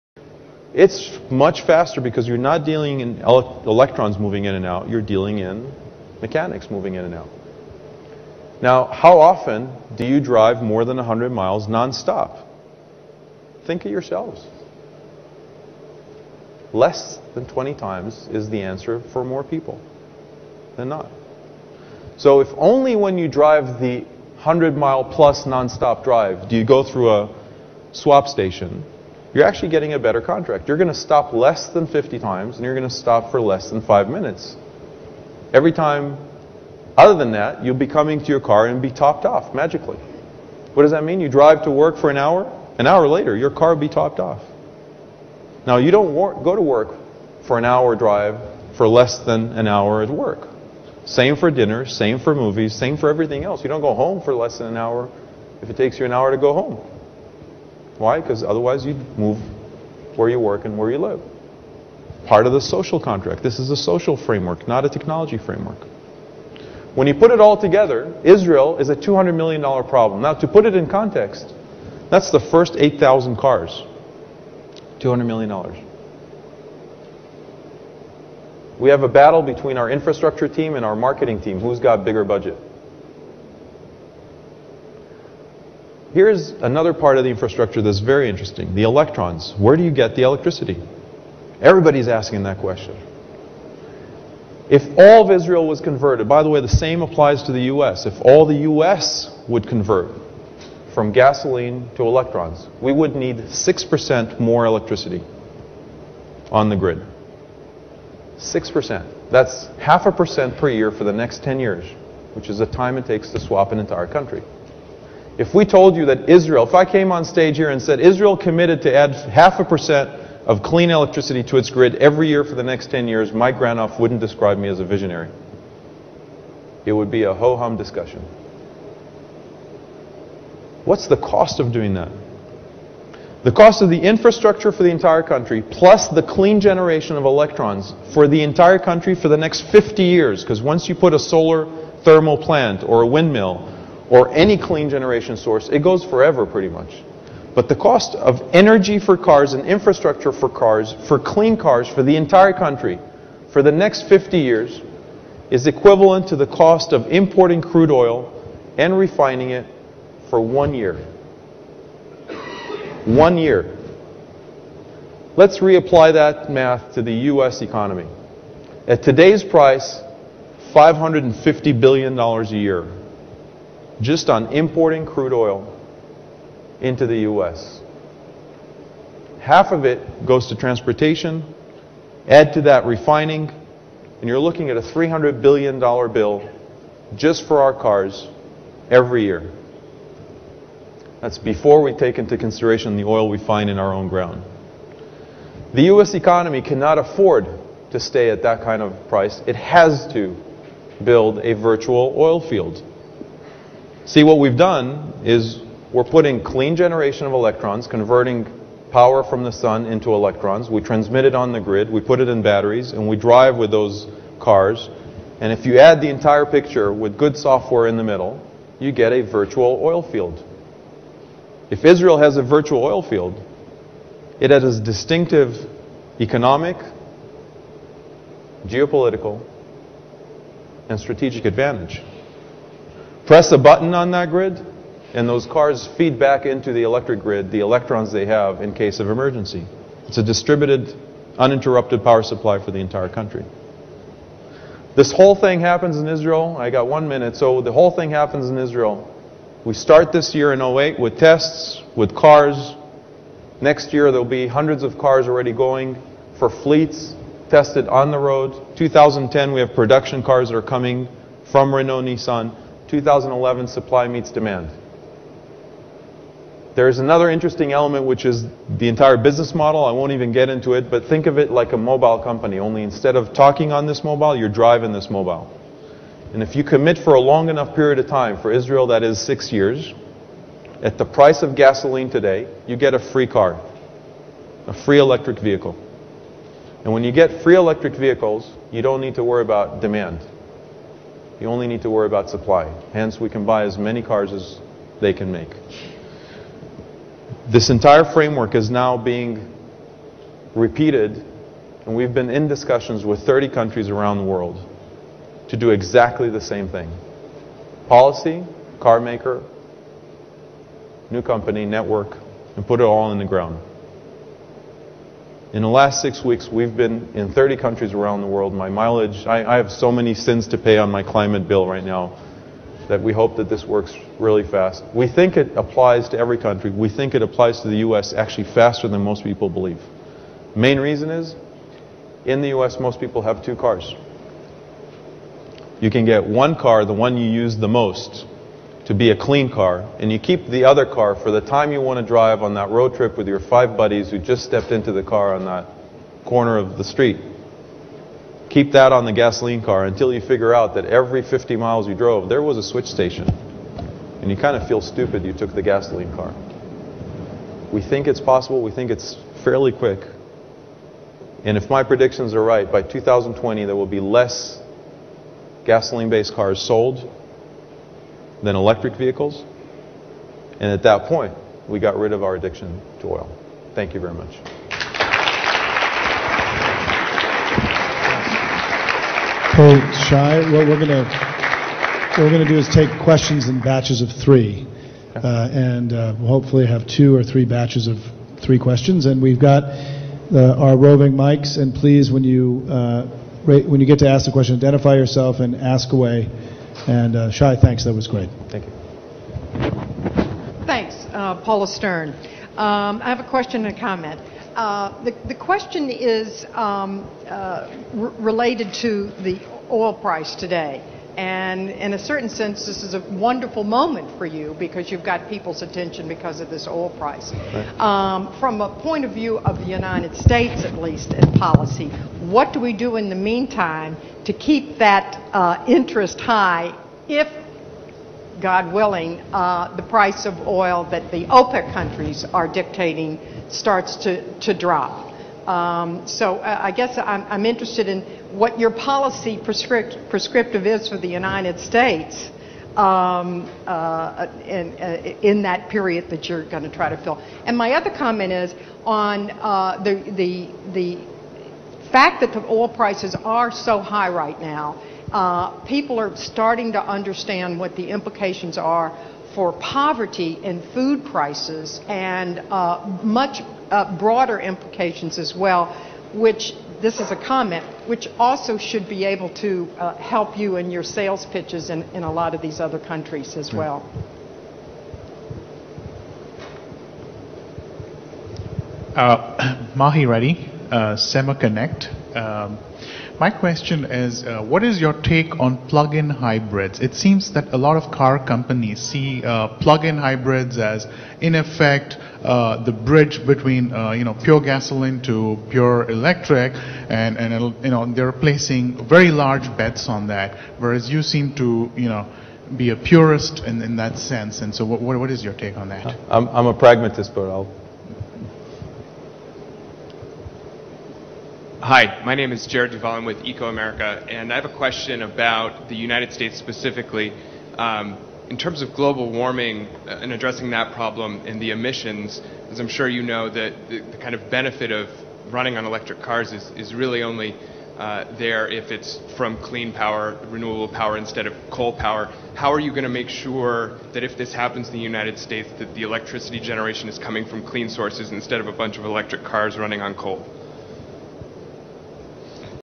Israel: The Electric Cars Project - part 3/5
Shai Agassi is the founder and CEO of Project Better Place, a company focused on the early 21st century's biggest challenge - a scaleable and sustainable personal transportation system. In this role, Agassi works directly with governments, finance, automobile manufacturers and technology companies to install, scale, and operate a regional and global infrastructure necessary for electric vehicles. With a personal passion in solving large-scale social and environmental issues, Agassi believes in utilizing technology and capital markets to address the challenges of sustainability and climate change. With Project Better Place, he will manage the operation of international electric vehicle fleets by investing in, installing and operating countrywide charging infrastructures for electric cars, as well as working with partners to make the cars available.